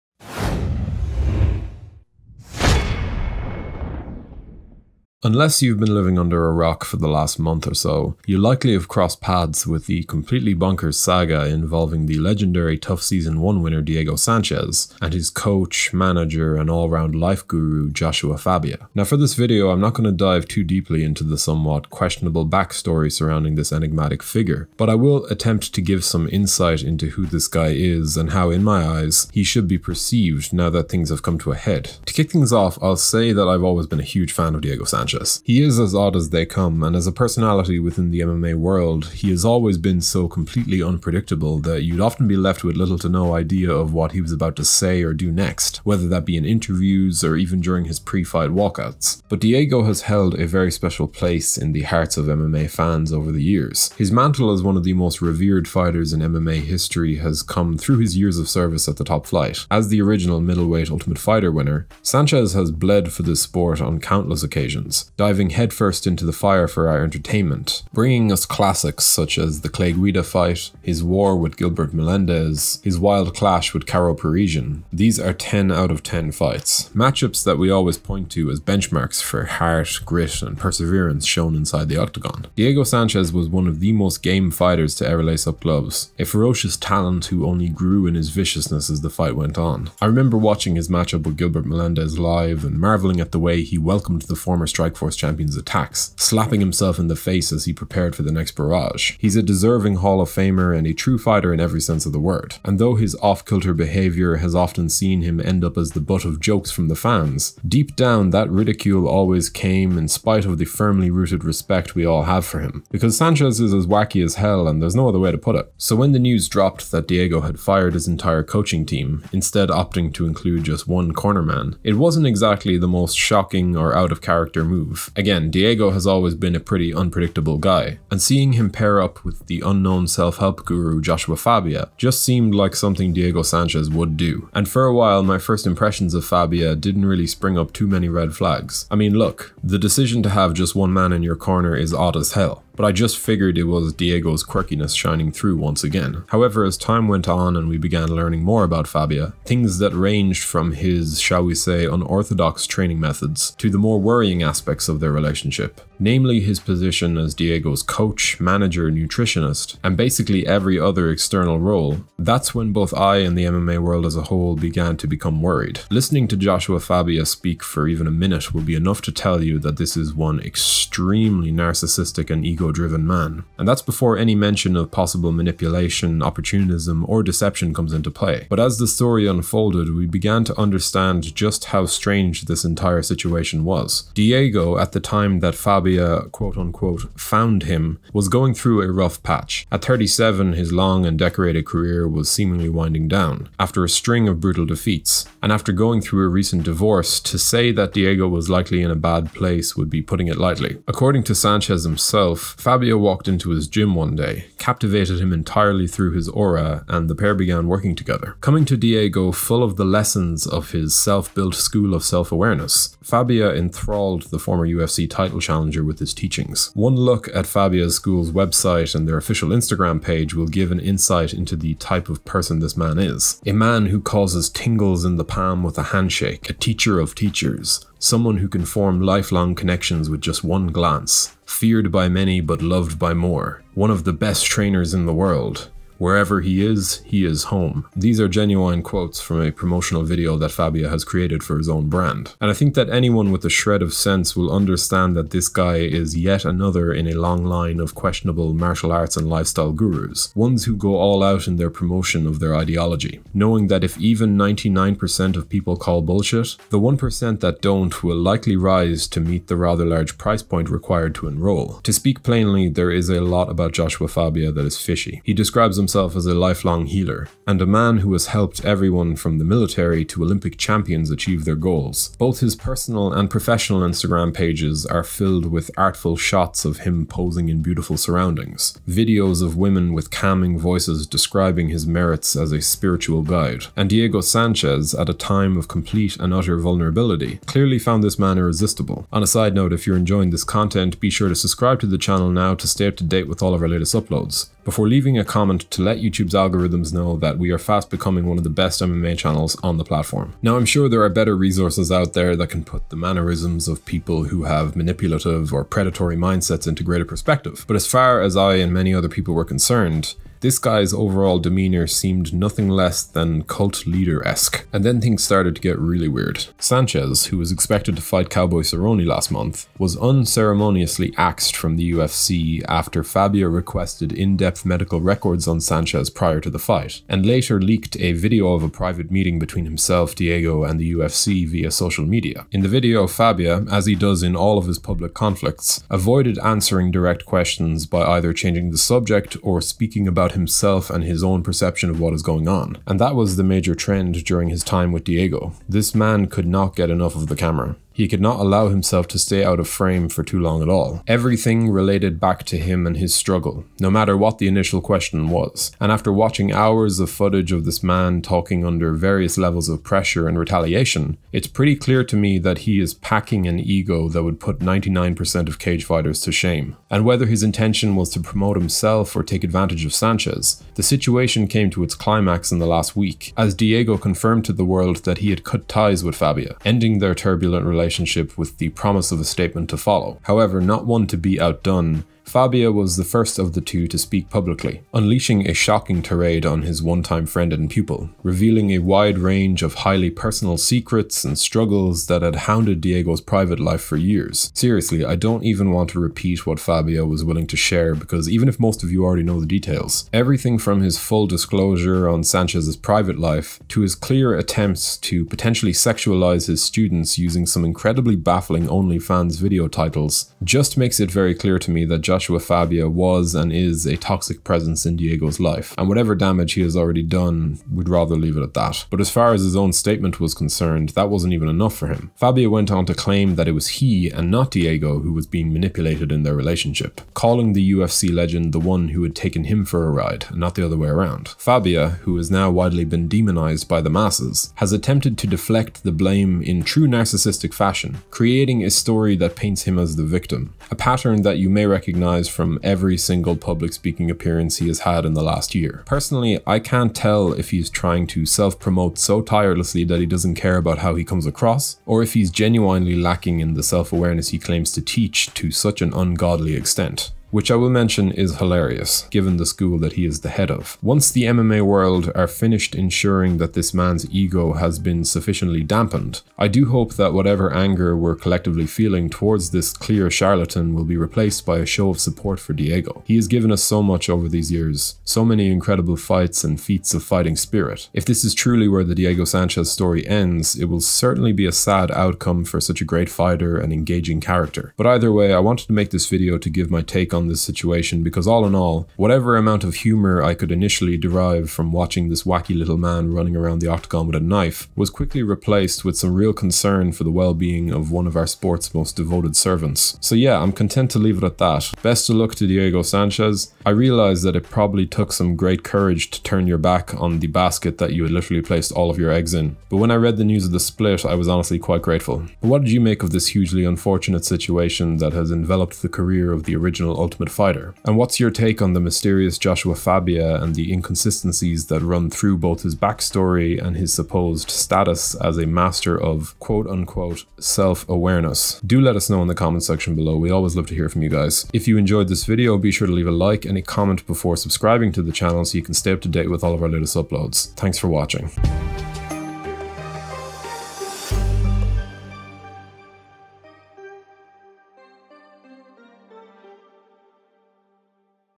The Diego Sanchez and Joshua Fabia Situation Explained
Unless you’ve been living under a rock for the last month, you’ll have likely crossed paths with the completely bonkers saga involving the legendary TUF season 1 winner Diego Sanchez and his coach, manager, and all-round life-guru Joshua Fabia.

Now for this video, I’m not going to dive too deeply into the somewhat questionable backstory surrounding this enigmatic figure - but I will attempt to give some insight into who this guy is, and how, in my eyes, this guy should be perceived now that things have come to a head.